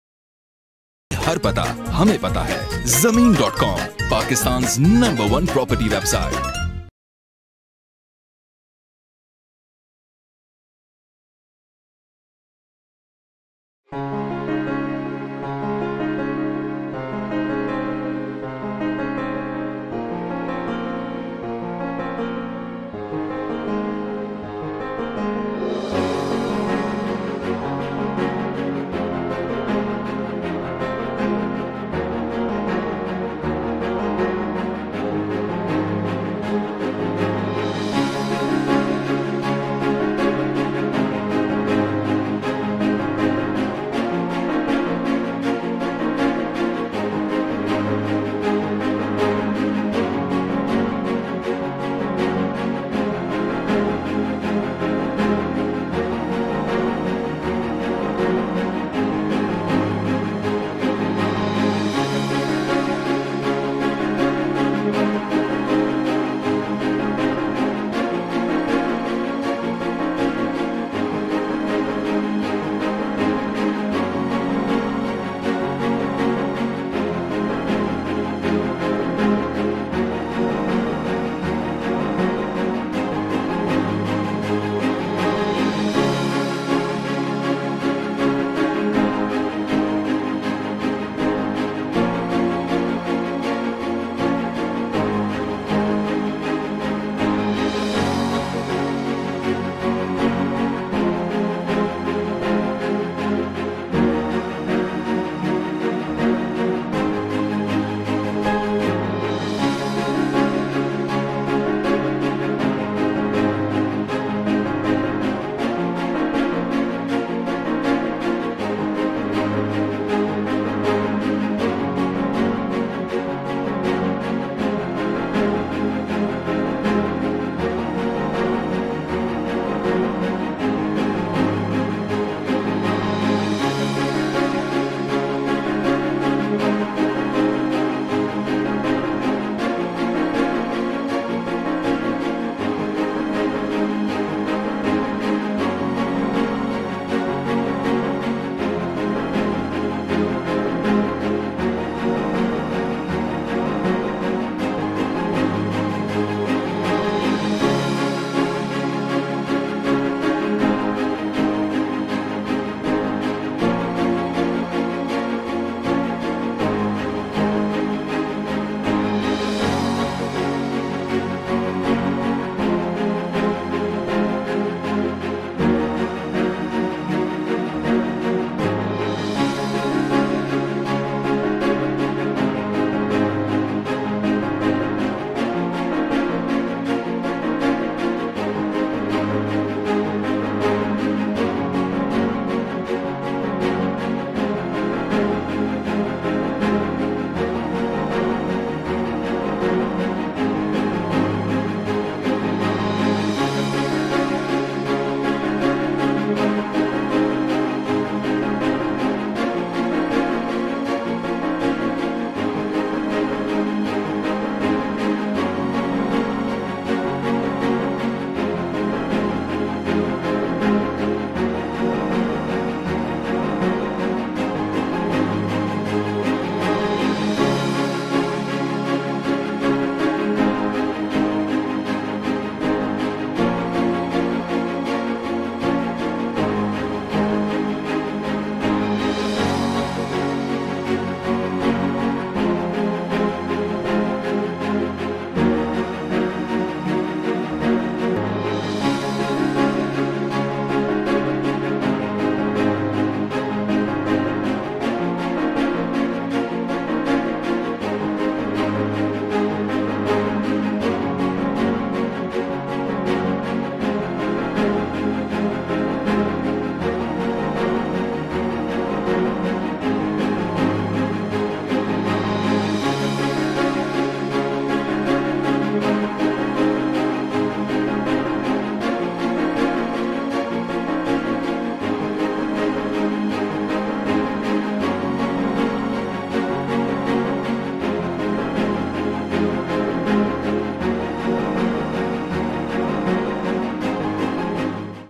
RESIDENTIAL PLOT FOR SALE IN BAHRIA SPORTS CITY BAHRIA TOWN KARACHI
Residential Plot For Sale
Bahria Sports City, Bahria Town Karachi, Karachi
500 Sq yard in Sports City
Available for sale
percent=37
Advance =661,000
installment =325,000,4
Transfer fee=55,000
own Money =6100000
Total Amount= 2616000..